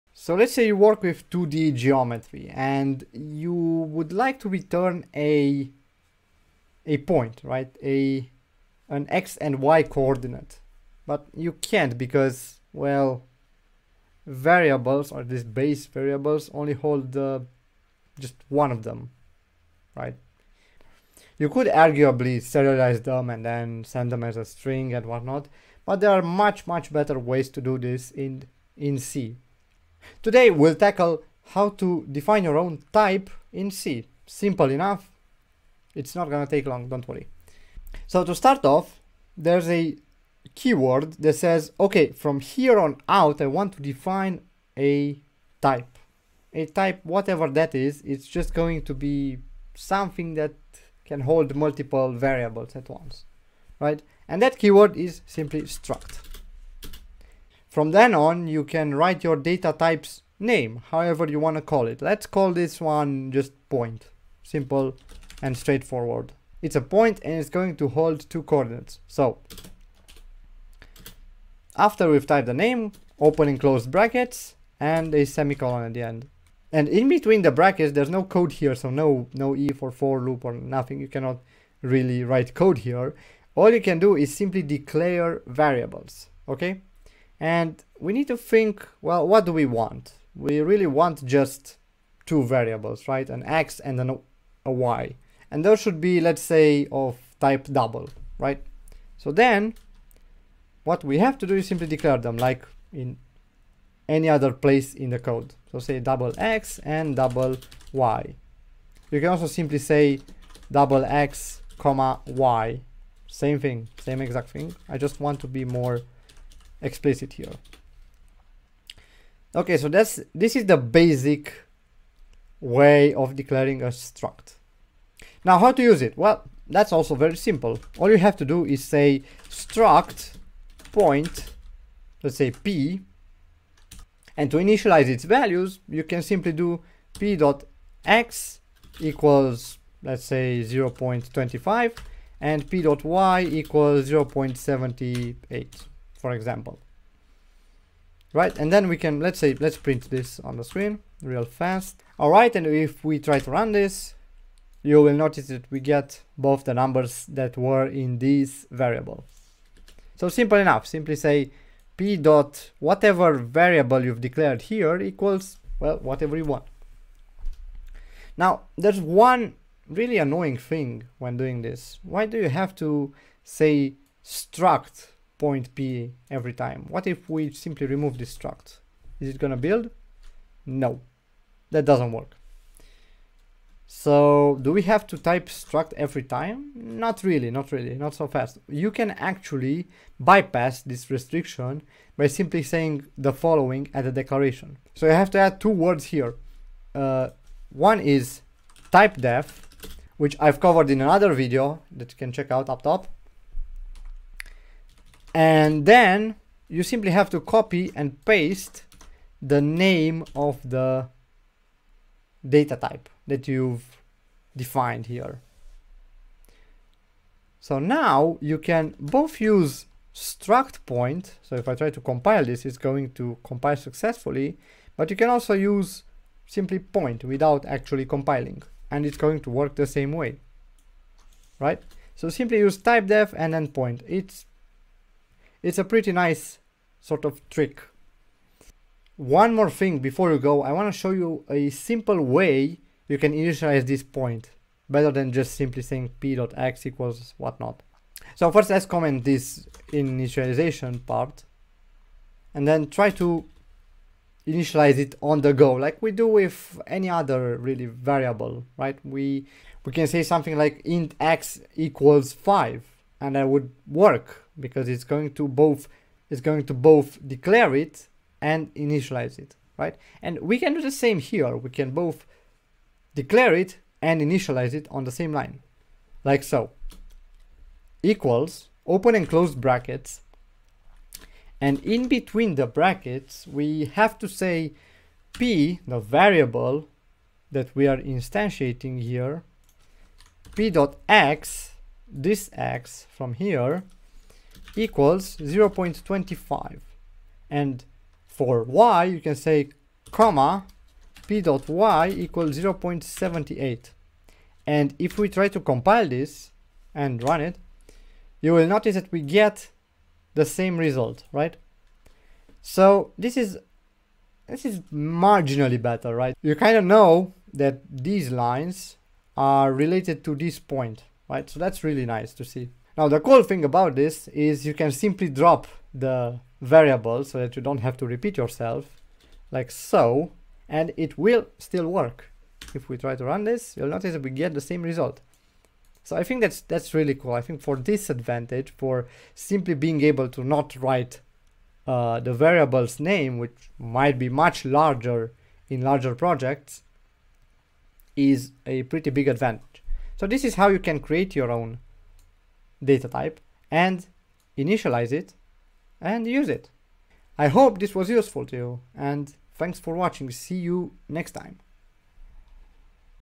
Creating your own data type in C
Ever wanted to customize C? Or simply return multiple variables at once? This is the way to do it!